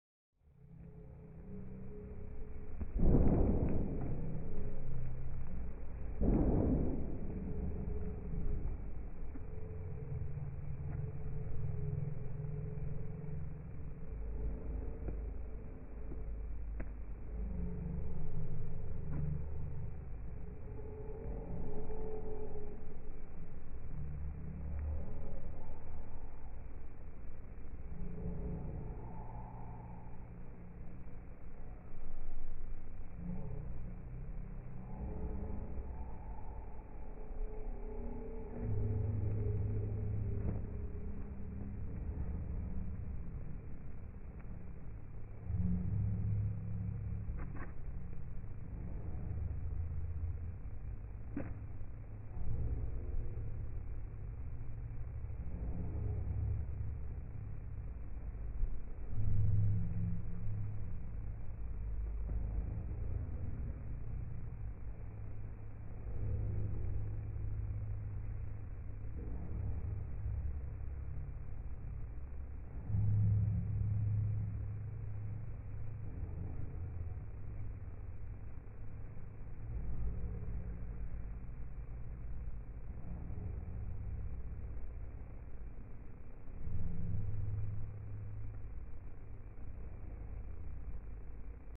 Slomo Siamese Misty Zen
Siamese Misty hits Zen over the scratch pad in slow motion.